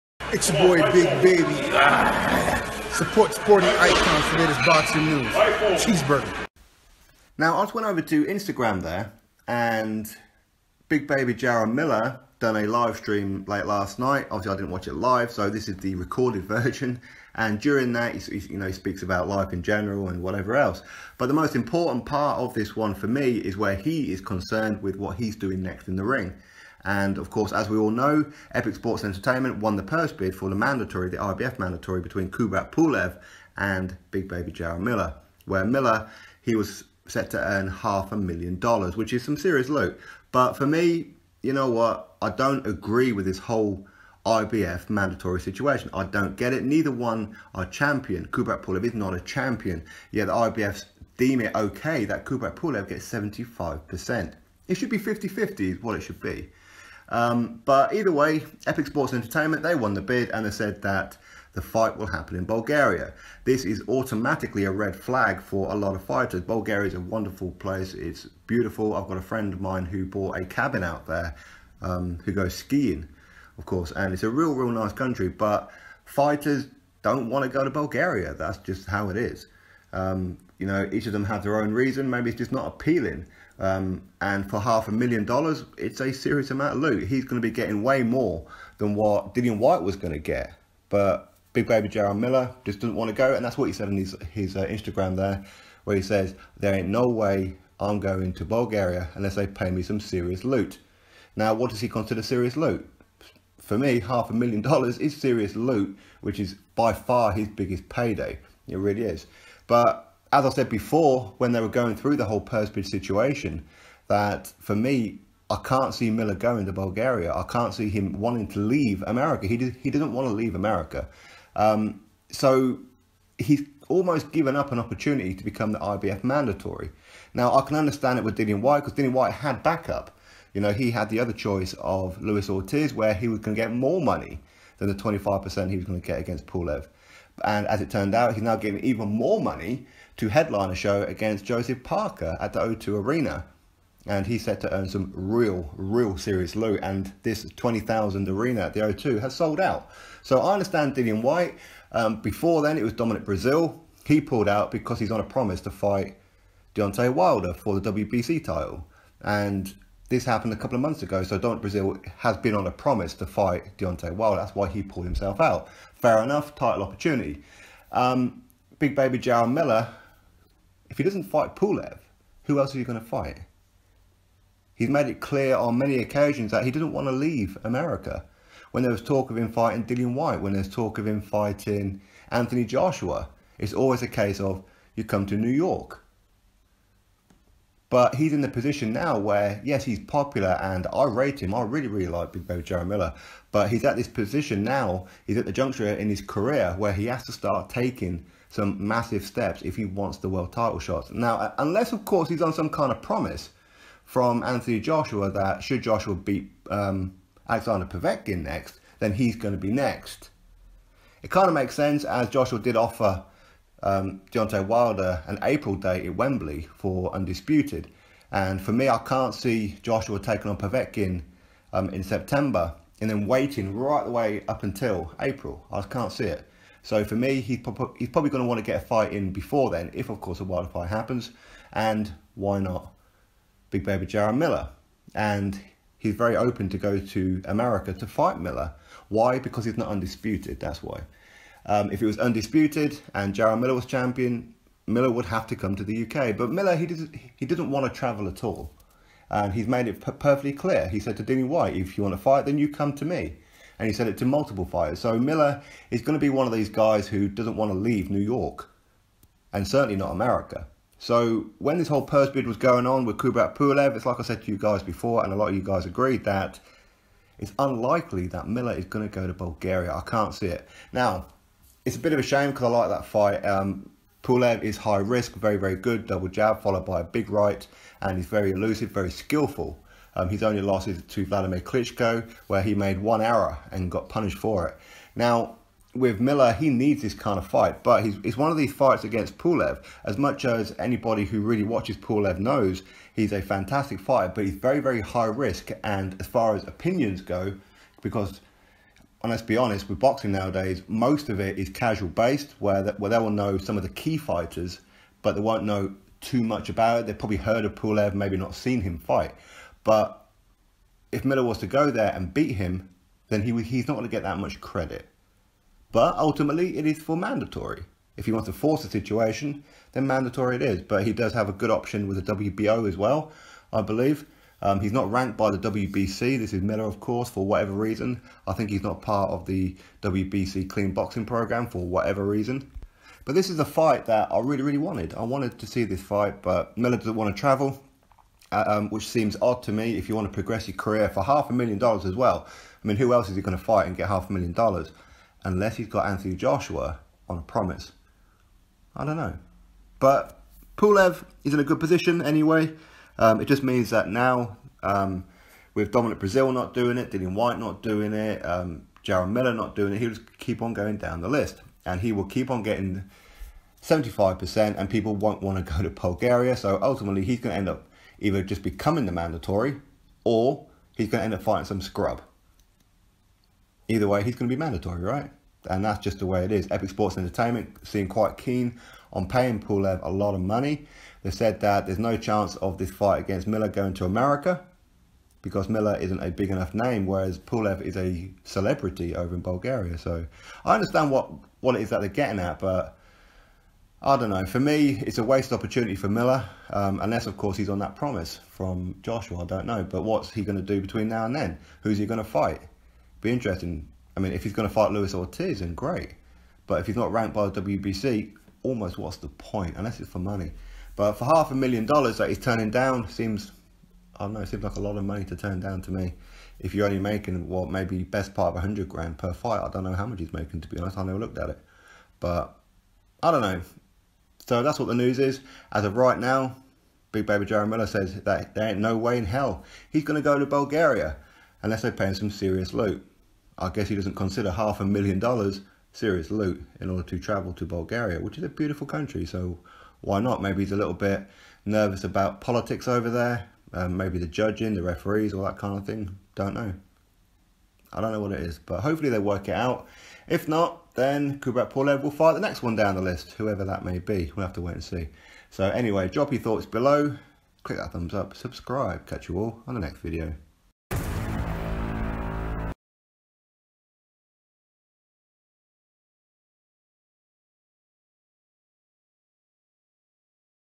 JARRELL MILLER REFUSES TO FIGHT KUBRAT PULEV IN BULGARIA!!
Big Baby Jarrell Miller refuses to travel to Bulgaria to face Kubrat Pulev for the IBF Mandatory!!